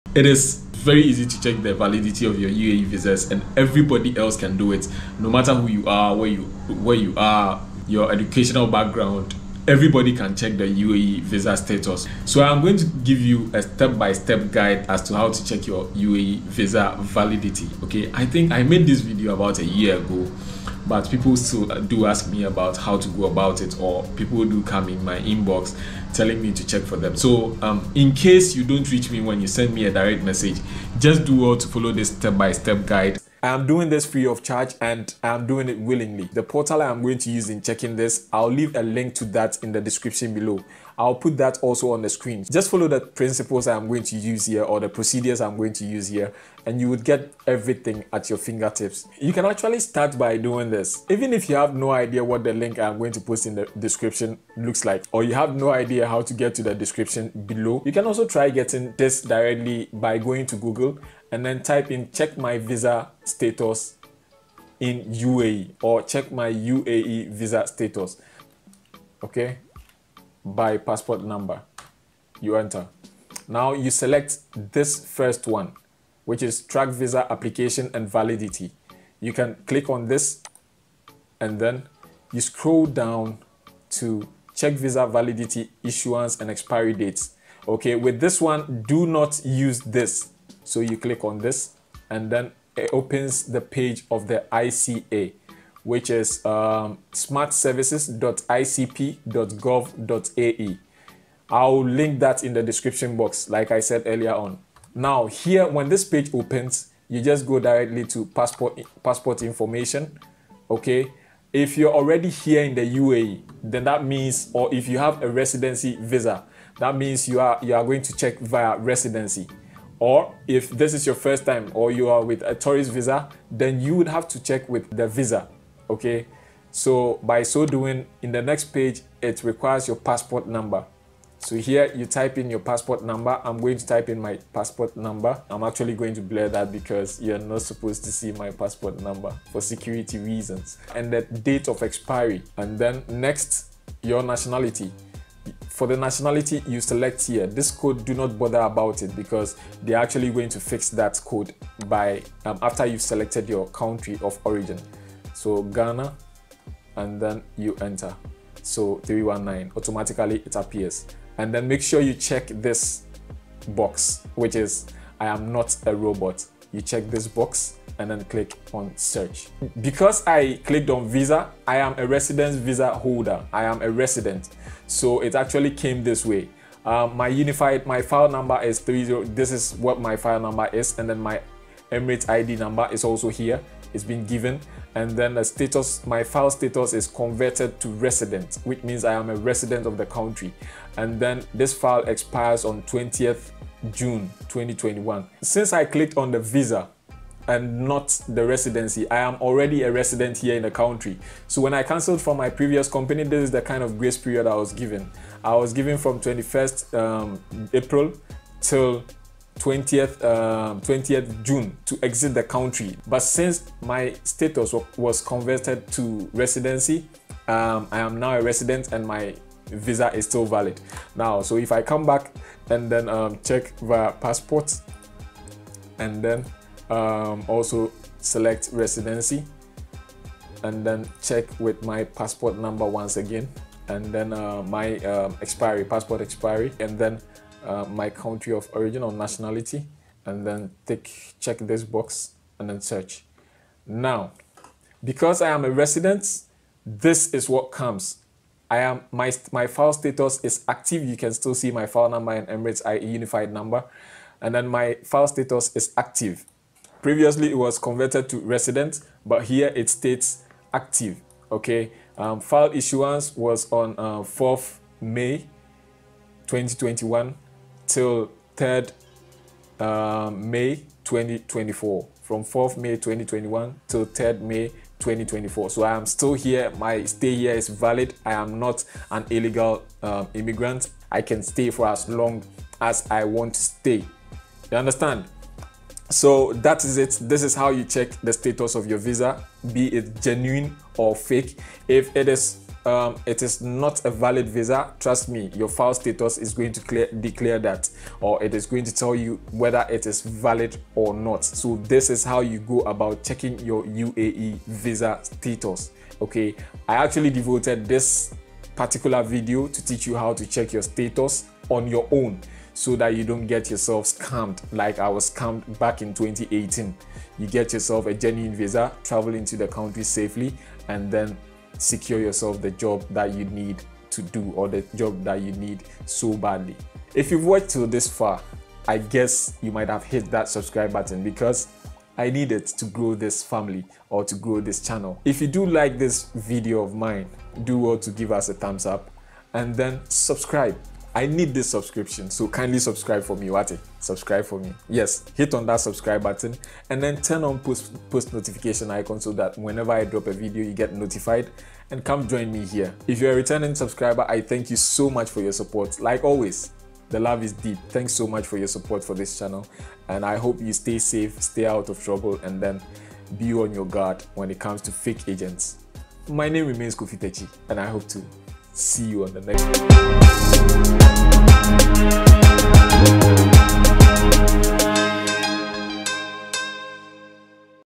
How to check Your Dubai VISA VALIDITY personally...
to avoid visa frauds, let me take you through a step by step guide as to how to check the vaalidity of your UAE Visas from home and even in the Country...

welcome to the channel of truth..